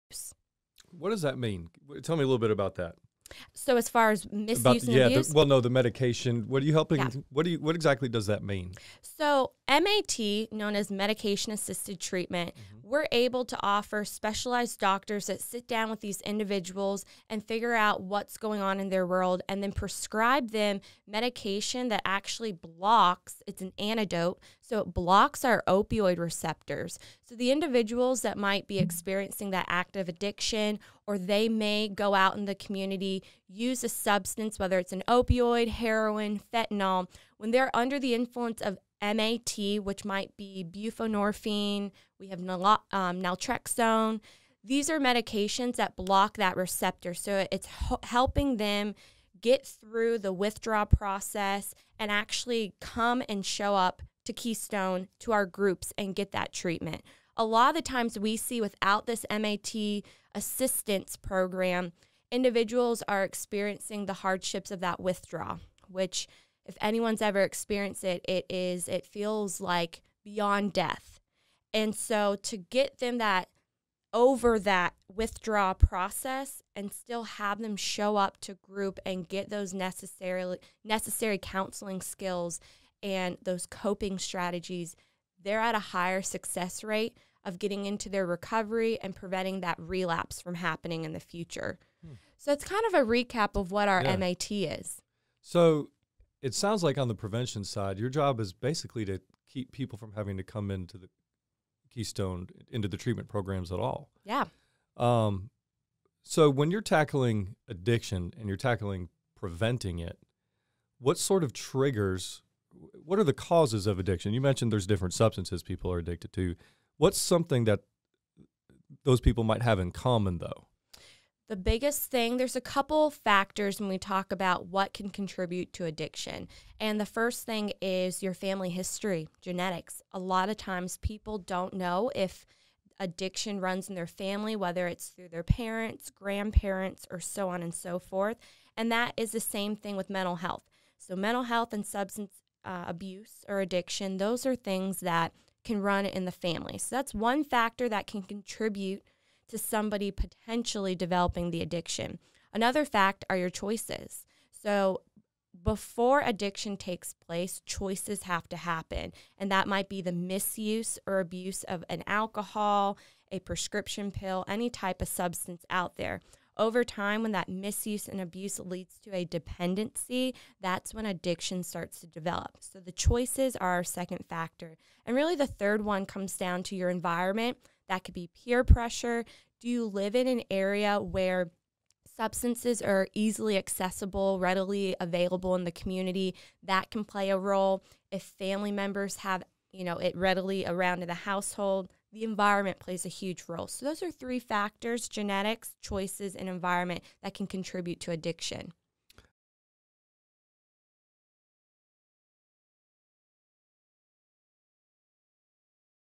Medication-Assisted Treatment And Factors Of Addiction Explained
Alex talks about the MAT program offered by Keystone and discusses 3 factors that lead to addiction.